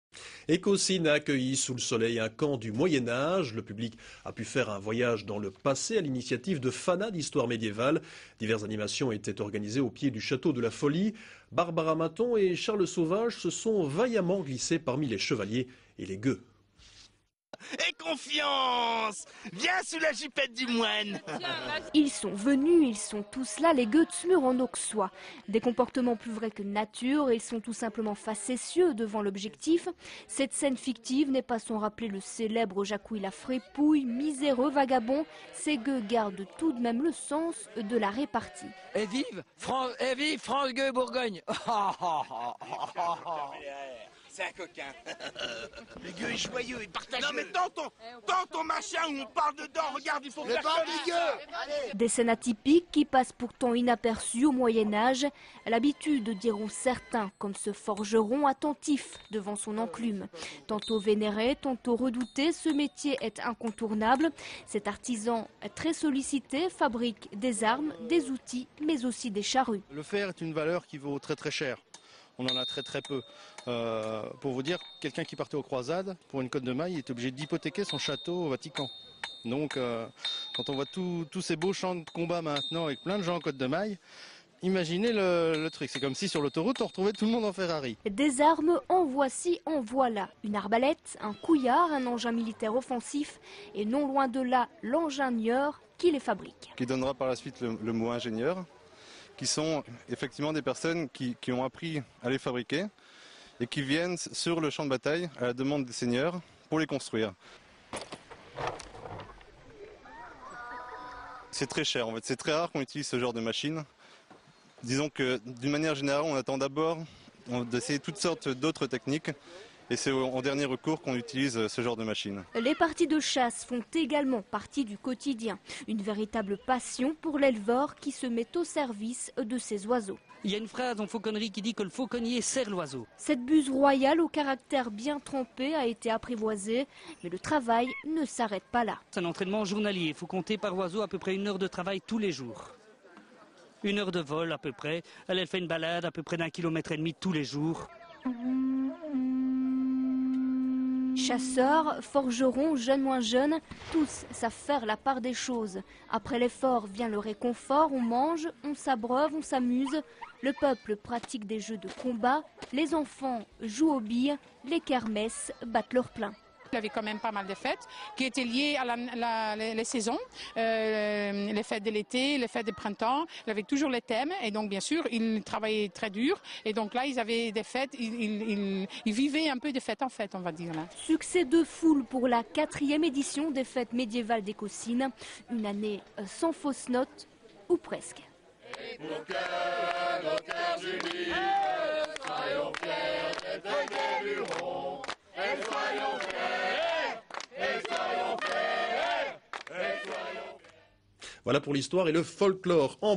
4EMEMEDIEVAL.wmv
4ème médiévale des EcausArts au Château de la Follie filmé par ACTV